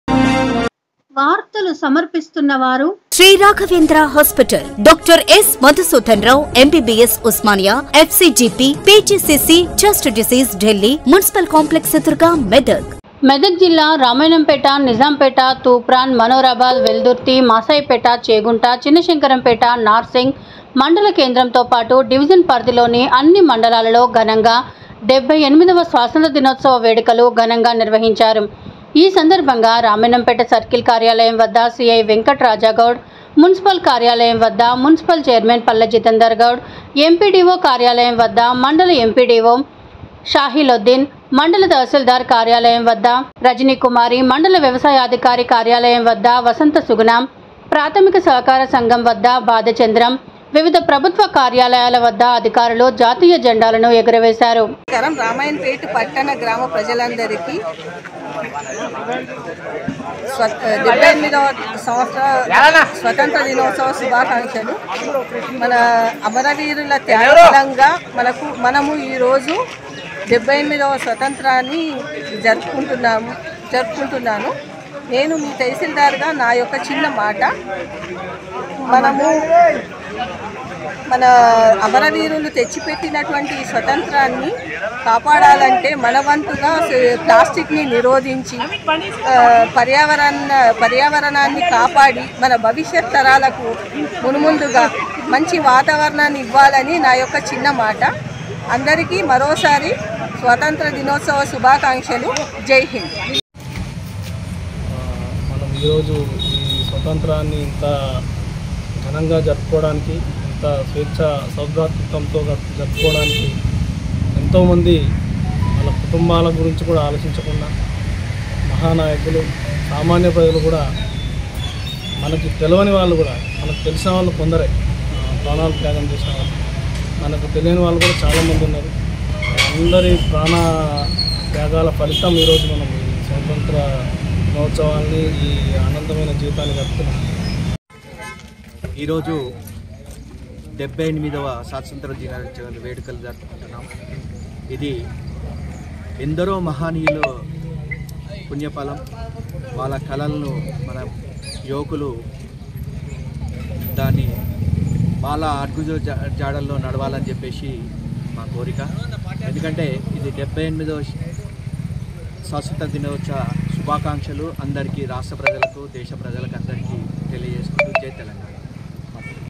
రామయంపేట అన్ని మండలాలలో ఘనంగా 78వ స్వాతంత్ర దినోత్సవ వేడుకలు
మెదక్ జిల్లా రామయంపేట  నిజాంపేట తూప్రాన్  మనోరా బాద్ వెల్దుర్తి మాసాపేట చేగుంట చిన్న శంకరంపేట నార్సింగ్  మండల కేంద్రంతో పాటు డివిజన్ పరిధిలోని అన్ని మండలాలలో ఘనంగా 78వ స్వాతంత్ర దినోత్సవ వేడుకలు ఘనంగా నిర్వహించారు